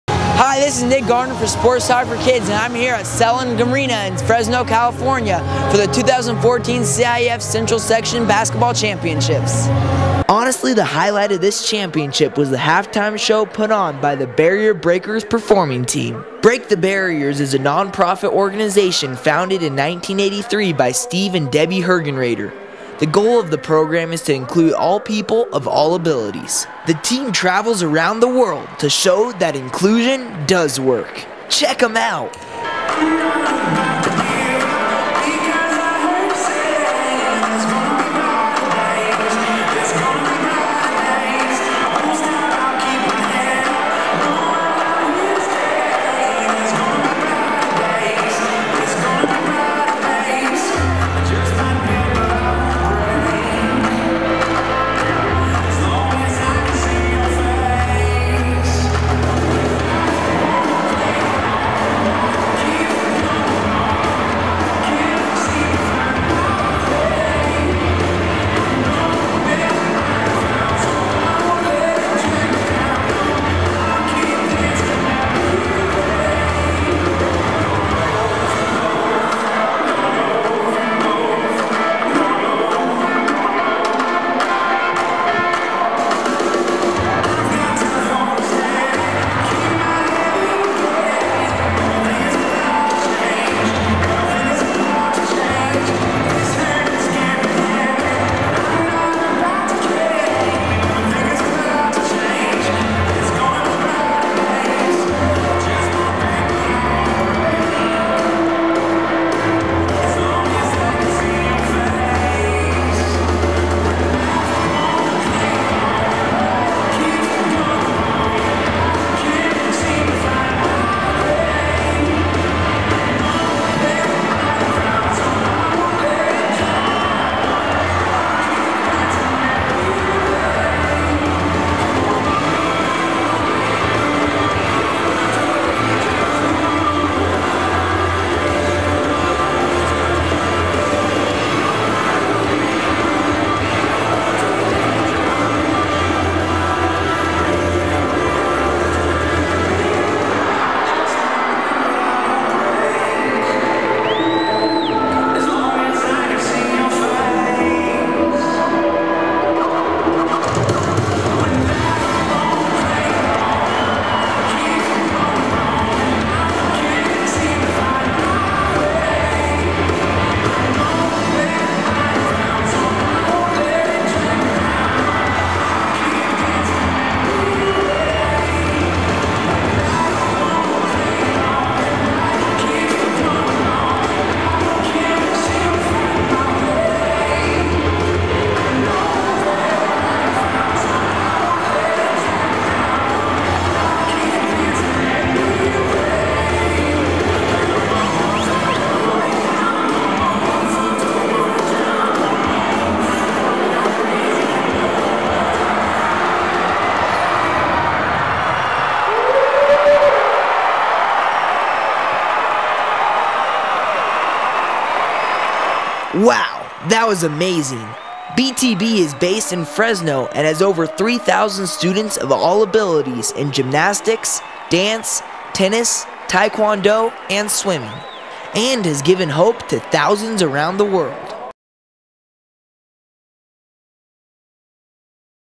Break the Barrier
Today, Nick Garner of SportsTalk4Kids was at Championship Weekend in the CIF Central Section Basketball Championships at Sellund Arena in Fresno, California.  Nick was there to watch basketball games, but the highlight of the day was the halftime performance of the championship game by the Barrier Breakers Performing Team.  Break the Barriers is a non-profit organization that travels around the world, a team of acrobats, dancers, and gymnasts, some with disabilities, others without, but a team that works together to send a message of inclusion, of perseverance, of strength, and of courage.  Break the Barriers consists of over 3,000 students of all abilities in gymnastics, dance, tennis, Tae Kwon Do, and swimming.  By teaming adults and children, able-bodied and disabled, Break the Barriers shows that we are all the same, just in different bodies.